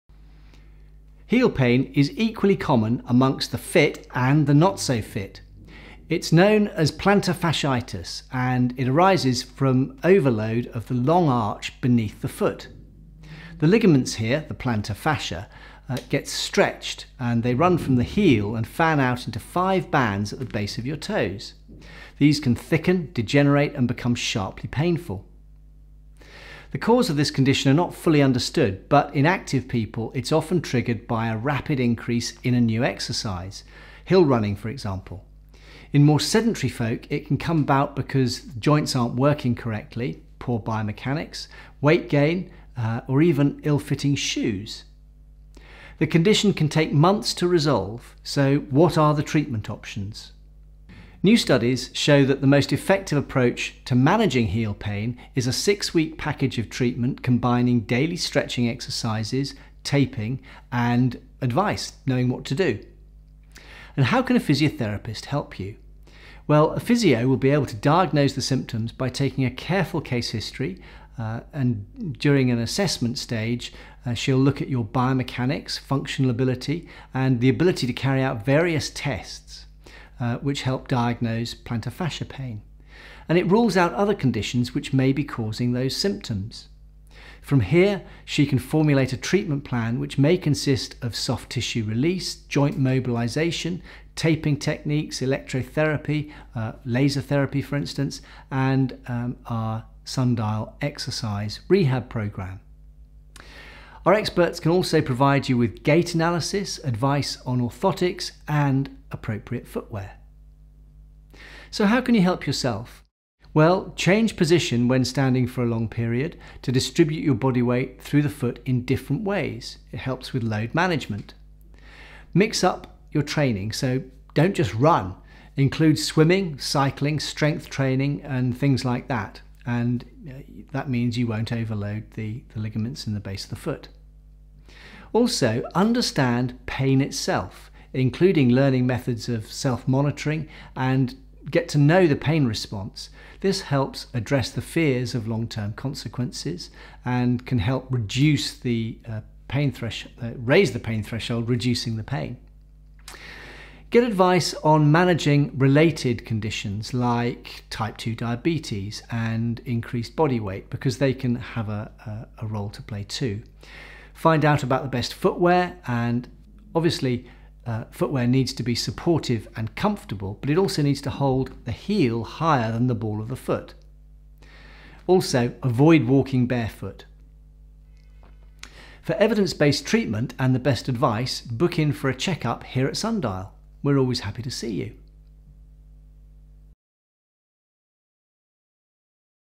How to cure heel pain
Heel pain is equally common among the fit and the not-so-fit. It’s known as Plantar Fasciitis, arising from overload of the long arch beneath the foot. The ligaments (Plantar Fascia) that run from the heel fan out into five bands to the base of your toes, and these can thicken, degenerate and become sharply painful.

It’s often triggered by a rapid increase in a new exercise (hill running, for example). Or it can come about because joints and muscles aren’t working correctly (poor biomechanics), weight gain or even ill-fitting shoes. The condition can take months to resolve. So what are the treatment options?

New studies show that the most effective approach to managing heel pain is a six week package of treatment, combining daily stretching exercises, taping and education.  Watch to find out more.